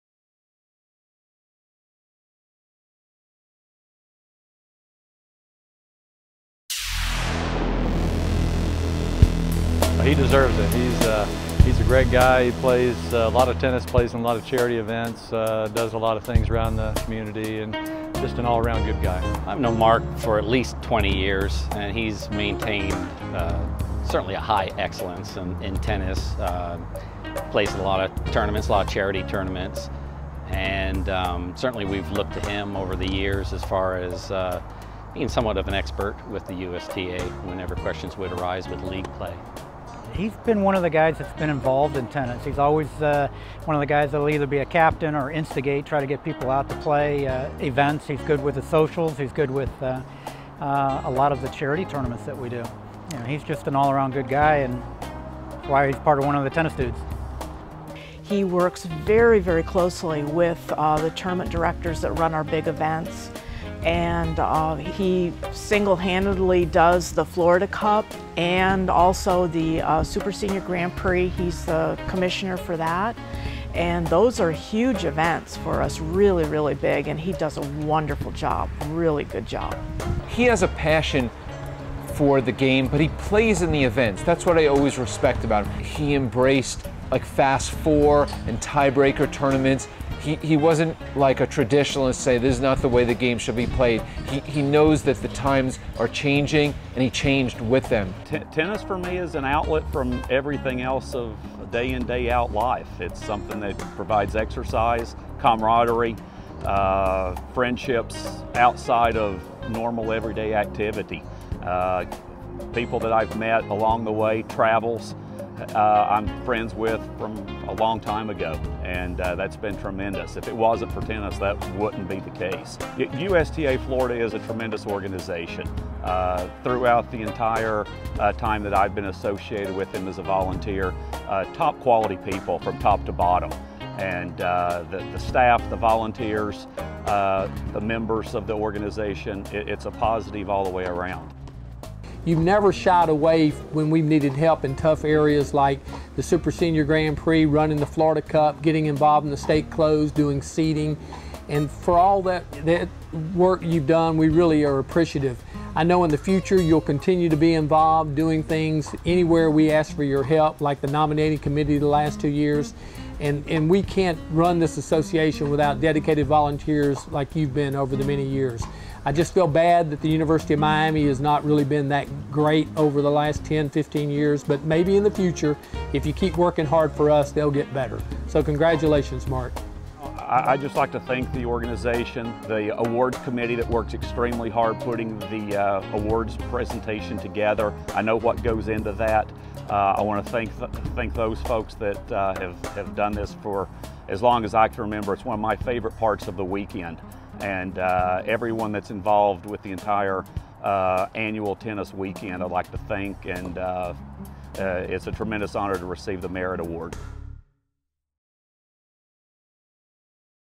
2016 Merit Award: Mark Taylor
Mark is an example of an adult competitive player who turned his love of the game into a mission to make tennis more enjoyable for his fellow adult competitors. He has done so by serving on Florida Section councils and committees ranging from competitive to USTA League to rankings, budget and finance, and more.